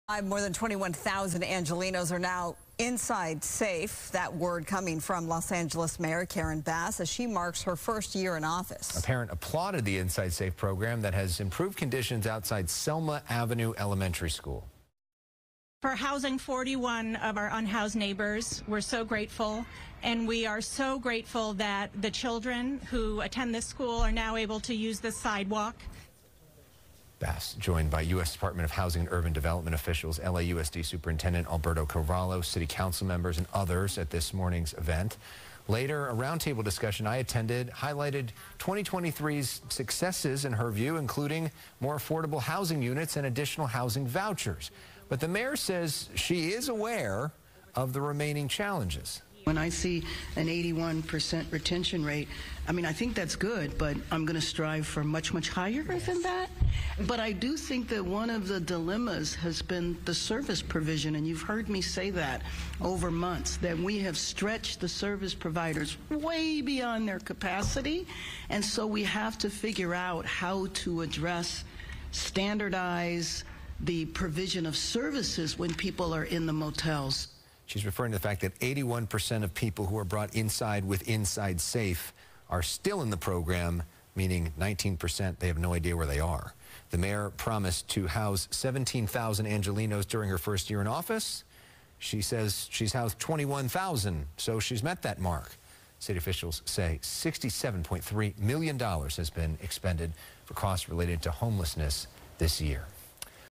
After her 1st year in office, LA homeless crisis still a priority for Mayor Bass
LA Mayor Karen Bass reflects on her first year in office.